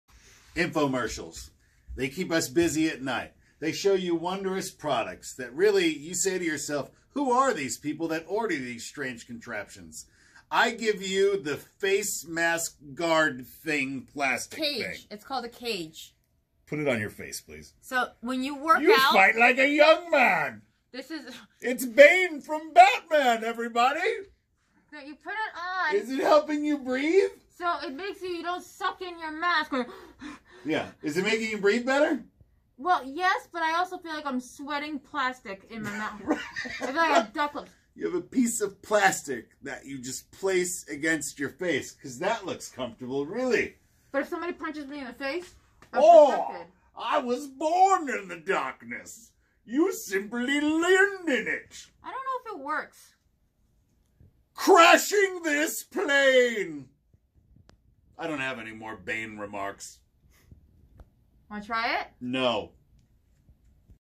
Infomercial Products
Anna tests out the mask bracket! A nifty little device that goes under your mask and makes breathing easier while exercising, allows you to speak more clearly, prevents "maskne" and more! Does it work though?